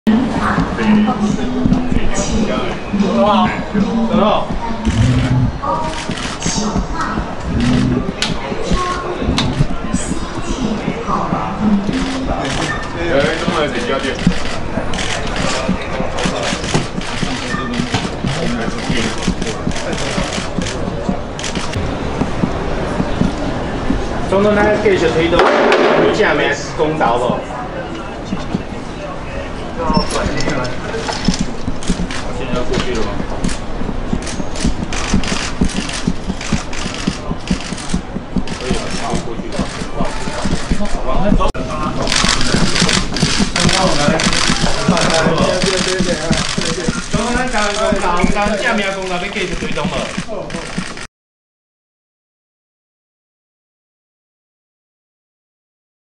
「台灣是不是要繼續推動正名」 李登輝：好好好
相關新聞連結:
高齡95歲的前總統李登輝伉儷今天下午搭機前往日本沖繩，李登輝此行受邀於那霸市日本台灣和平基金會邀請，將在6月24日出席「台灣人戰亡者慰靈碑」揭幕儀式，這也是是李登輝卸任後第9次訪問日本。